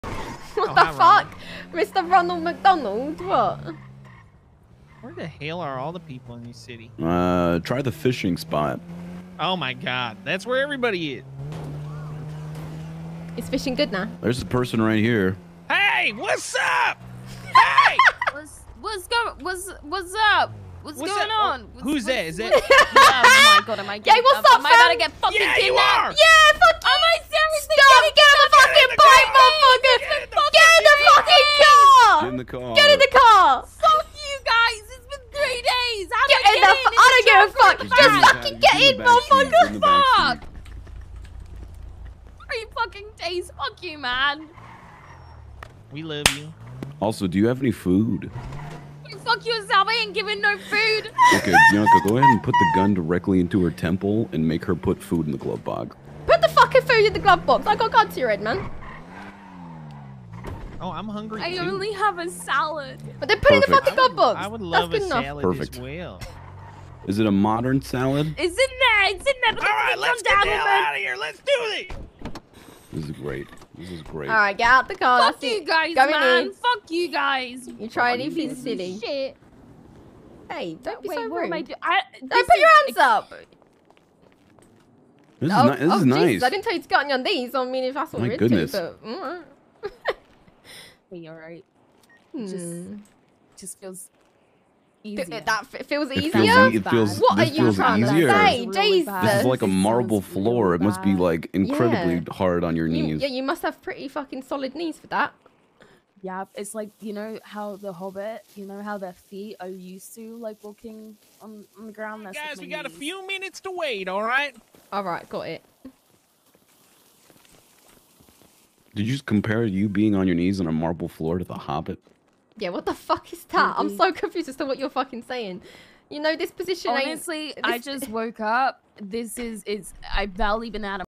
5 minutes after she wakes up, Cleo Shaw has her streak of 3 days without getting kidnapped broken by
5 minutes after she wakes up, Cleo Shaw has her streak of 3 days without getting kidnapped broken by the most unlikely group of criminals who haven't been in the city for months - Wolfabelle